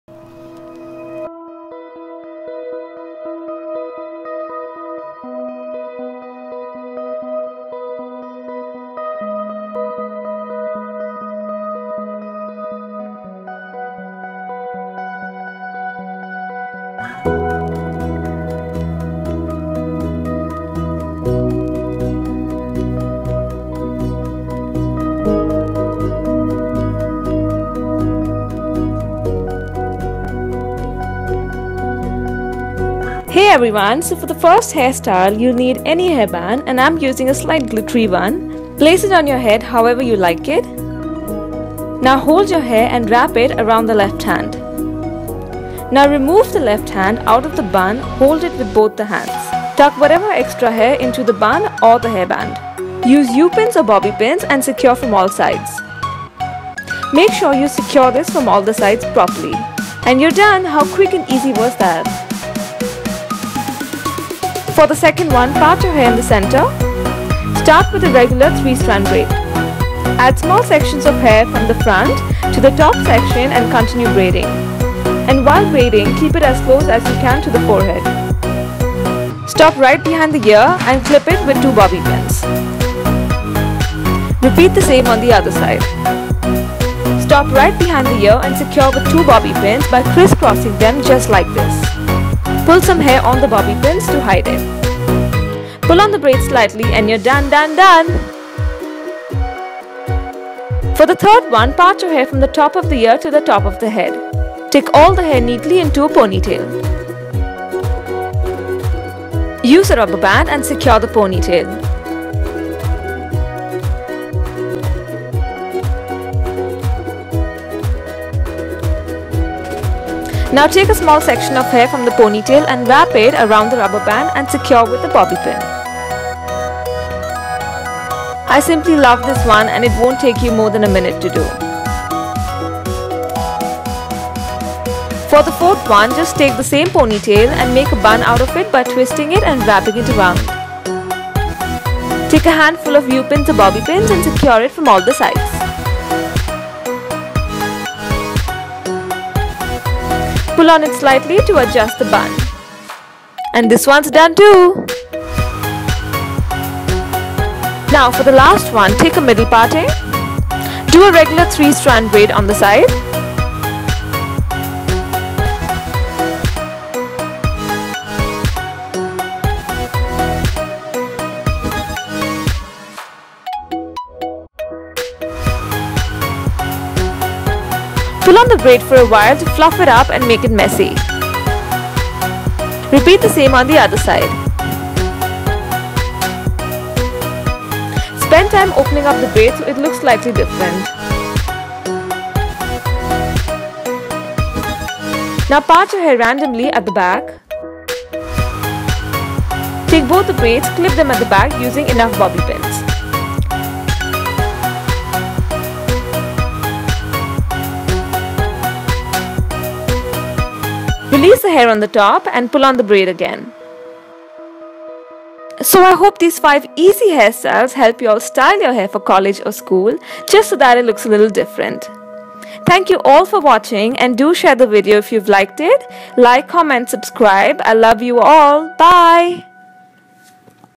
5 Everyday EASY Heatless Hairstyles\ Indian Hairstyles For Medium /Long Hair For School/College/Work
Get some inspiration to kickstart the year and transform your look by watching my new video on back to school hairstyles. Turn heads with these totally cool hairdos. Super easy and quick!

More Easy Braid Hairstyle videos-

3 Easy Headband Braids-

BRAID AWAY AND HAVE FUN MY LOVES!!  ♥ ♥ ♥